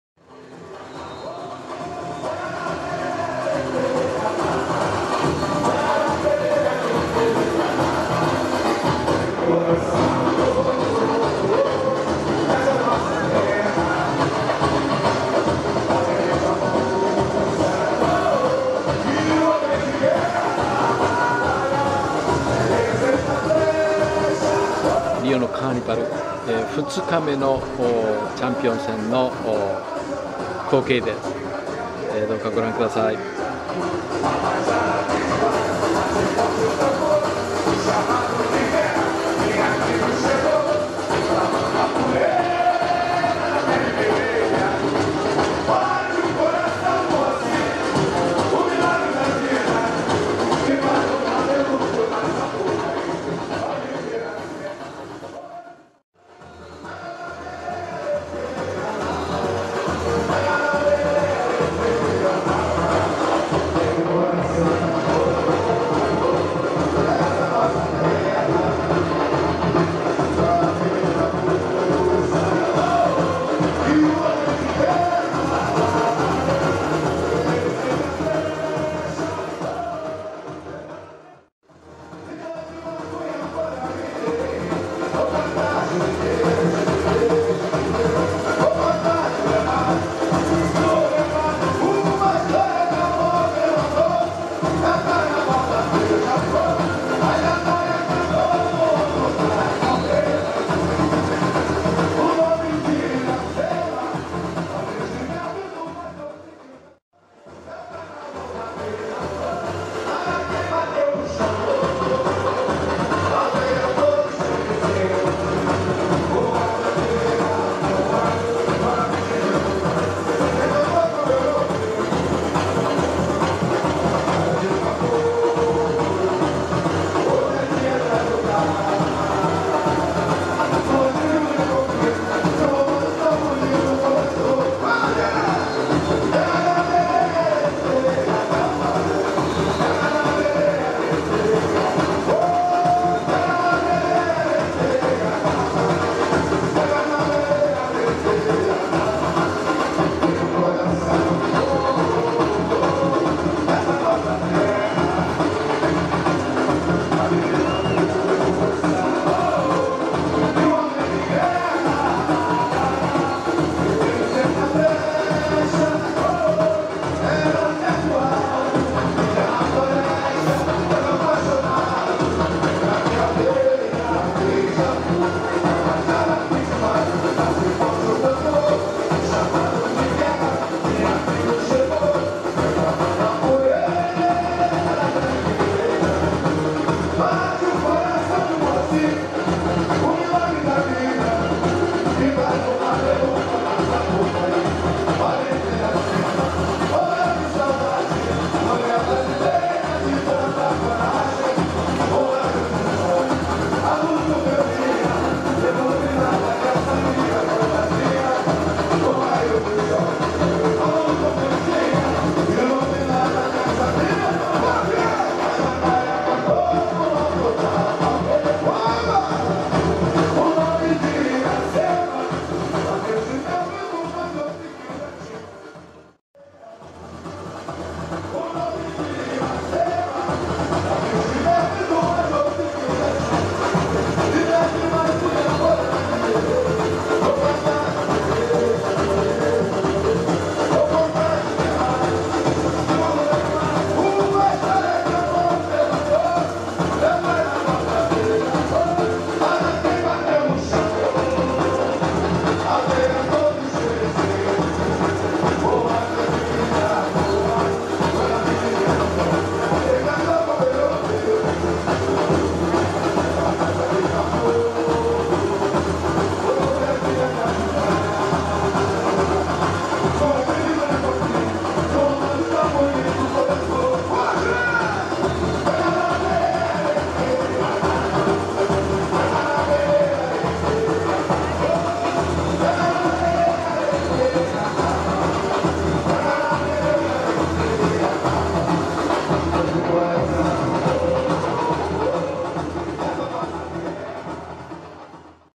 【旅エイター】ブラジルの旅『リオ・カーニバル２日目⑤ 写真集』 リオデジャネイロ
世界一のお祭り『リオのカーニバル』。会場に轟く大音響のサンバのリズム、目をひく大掛かりな山車、1チーム3,000人という踊り子の情熱的なダンス、収容70,000人の大スタンド。どれもがこの大パレードを盛り上げます。